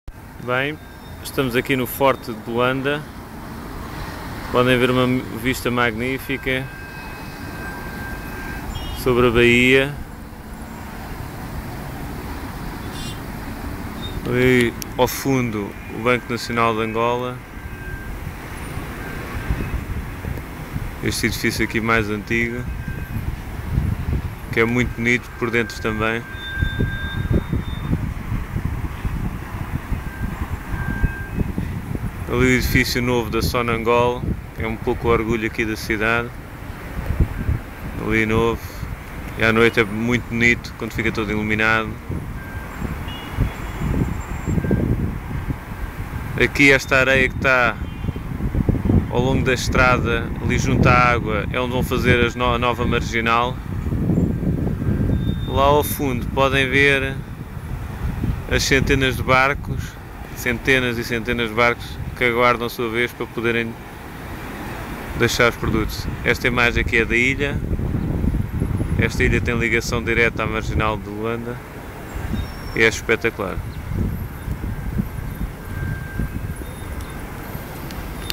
Vista Fortaleza de Luanda
Um Vídeo, na fortaleza de Luanda sobre a baía de Luanda